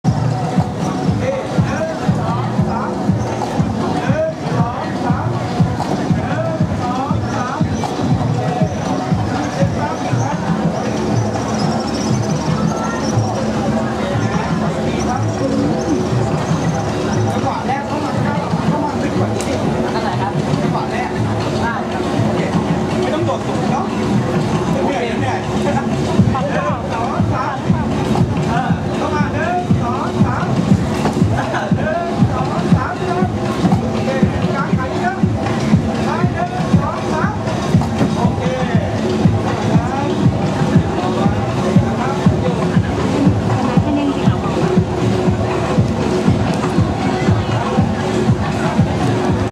Hunz - ถ่ายแบบ 3/4 Uniqlo @ Elle Fashion Week
ถ่ายแบบให้ Uniqlo @ Elle Fashion Week Fall/Winter 2015 at CTW